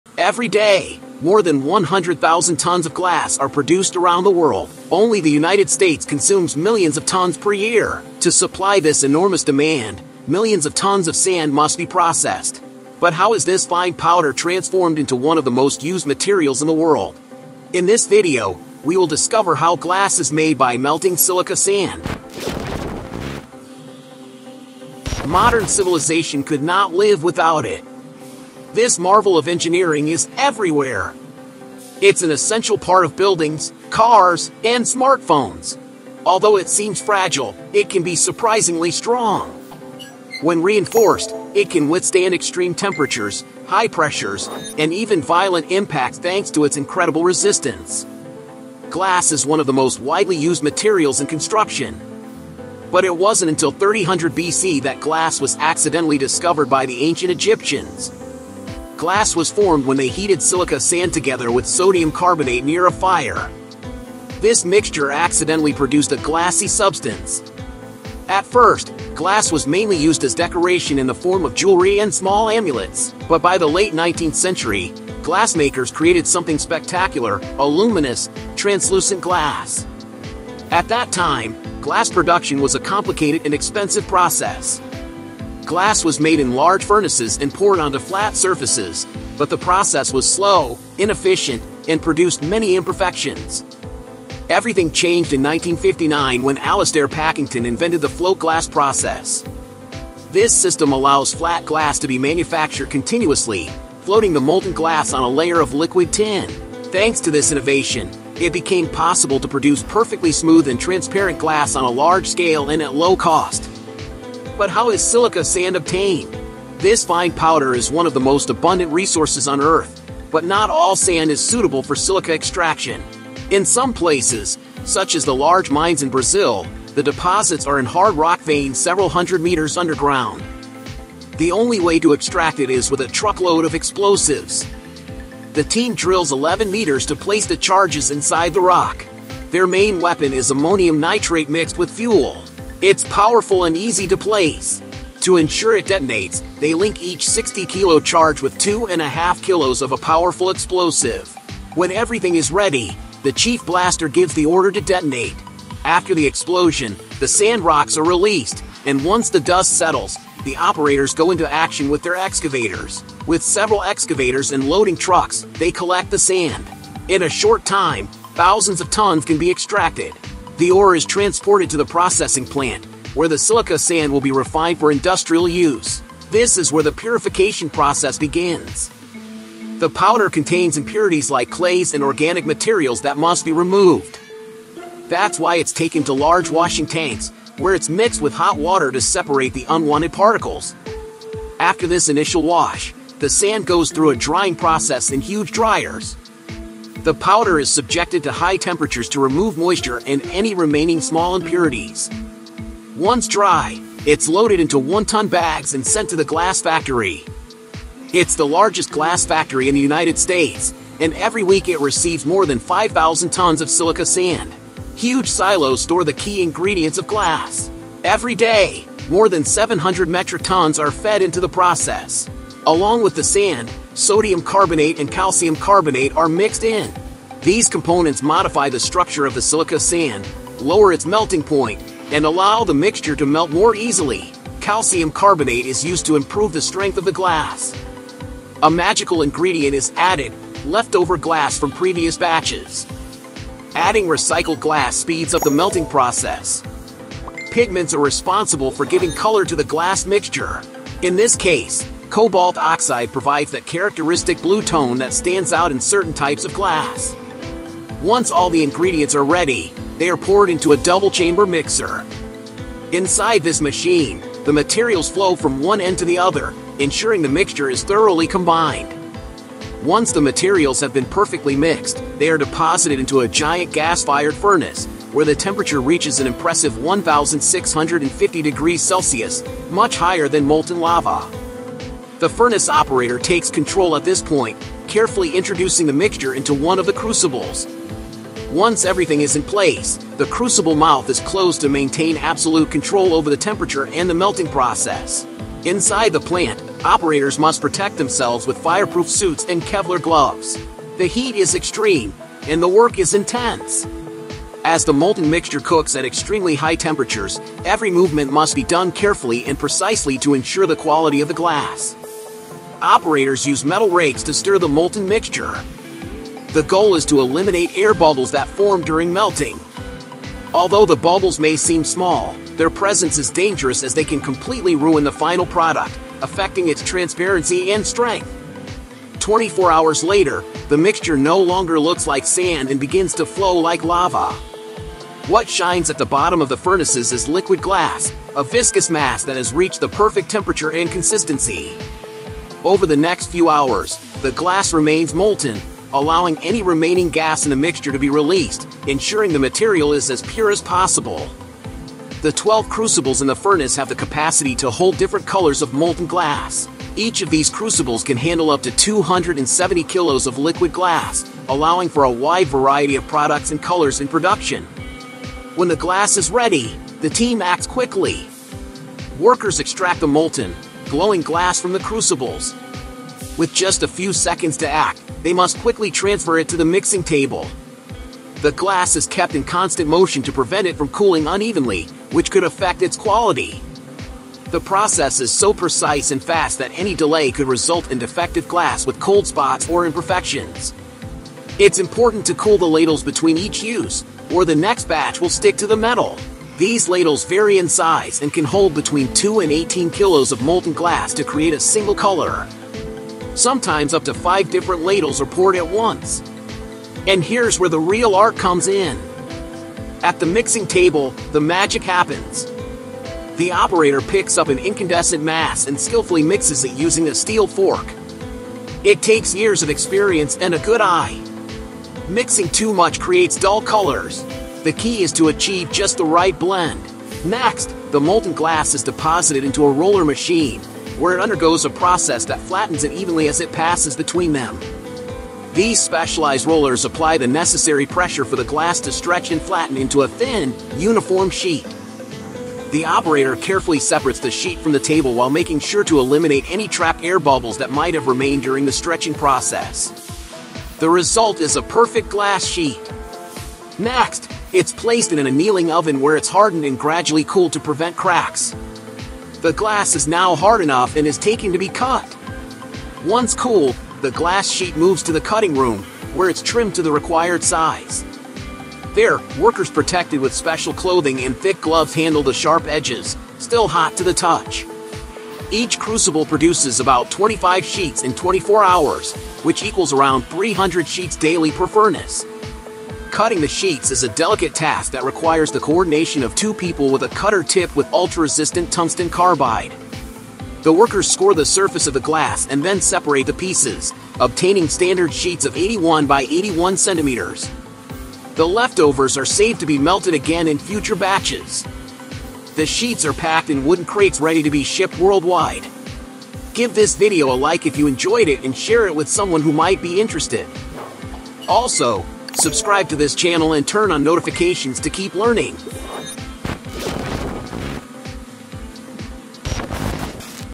🔥 How Glass Is Made from Sand
Every day, over 100,000 tons of glass are produced worldwide. But have you ever wondered how fine sand is transformed into one of the most widely used materials on Earth? In this video, we take you inside the fascinating process of making glass from silica sand, from mining and purification to melting at extreme temperatures and shaping it into perfect glass sheets. Discover the history, the technology, and the incredible craftsmanship behind every piece of glass you see around you — in buildings, cars, and smartphones. Watch until the end to see the full journey!